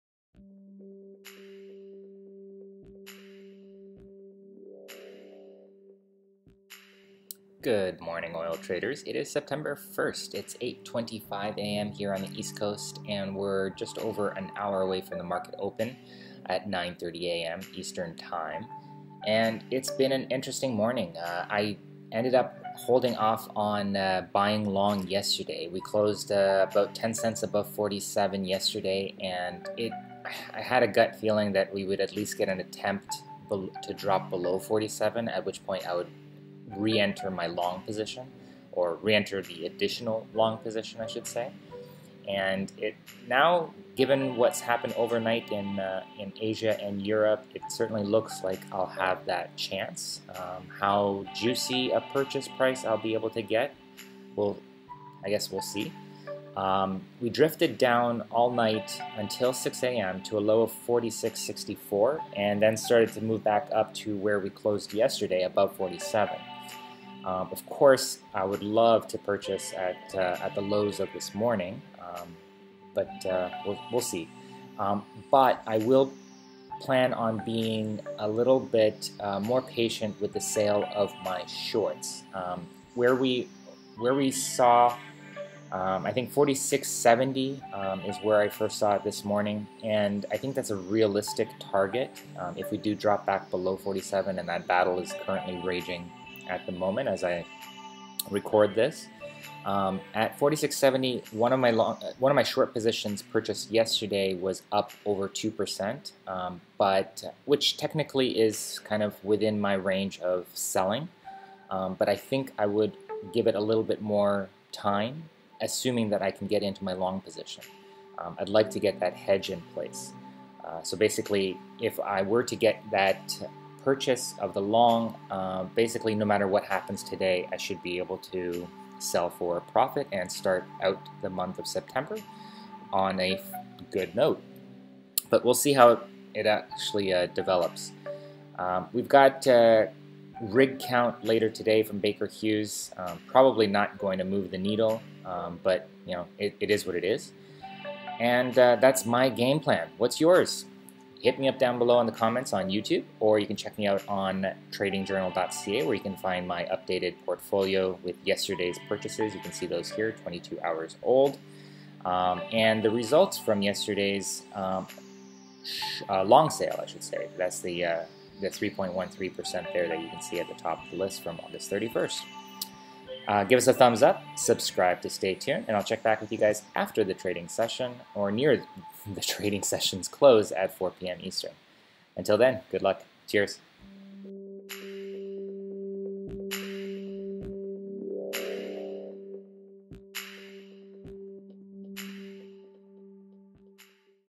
117 - Game Plan for Sept 1, 2017 - Looking to Hedge Yesterday's Shorts with a Long Buy sub-$47
* Crude drifted downward to a low of $46.64 overnight, but started to climb back to yesterday’s closing since 6am
* I was aggressive yesterday and if we do drop back below $47, I’ll keep up the pace
    * Planning on buying long sub-$47 - morning lows would be nice, of course :)
* I’ll be more patient with my Short sell orders - wait for Europe to close at 11:30a EST at least… probably